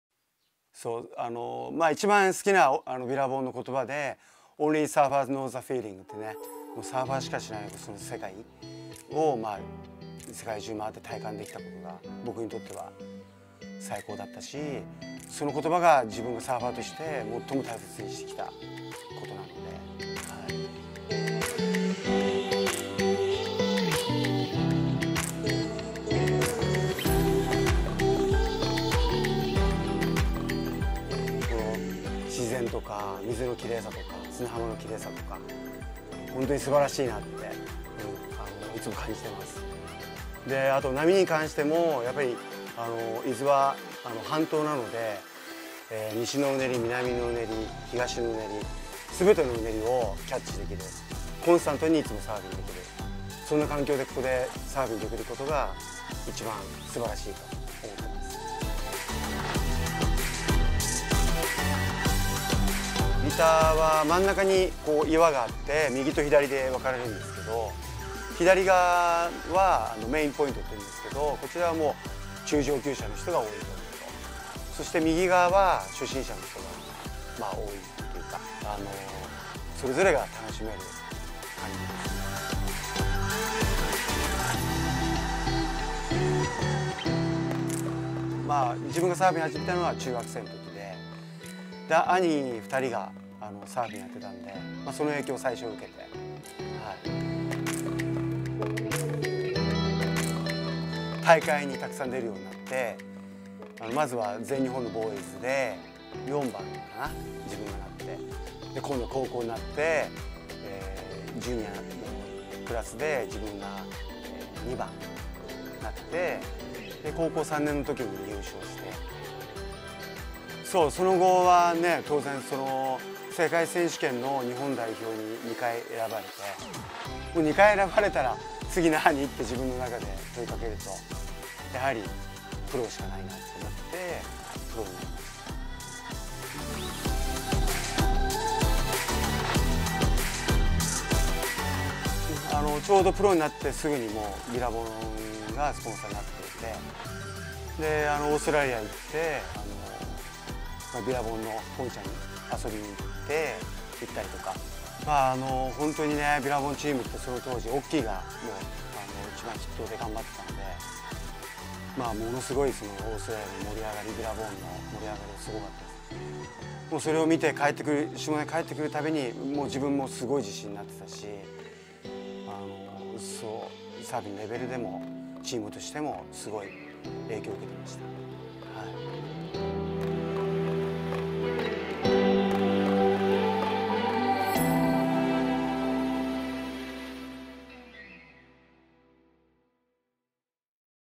BILLABONG CORE / Naoto Suzuki / Part 1
BILLABONG CORE Vol.2
Naoto Suzuki / Part 1
サーフカルチャーを育み続けている、海辺のボードメーカーやサーフショップをフューチャーするプロジェクト”ビラボンコア”に鈴木直人プロ出演パート1。
カラーズマガジン/インタビュー/パート1記事掲載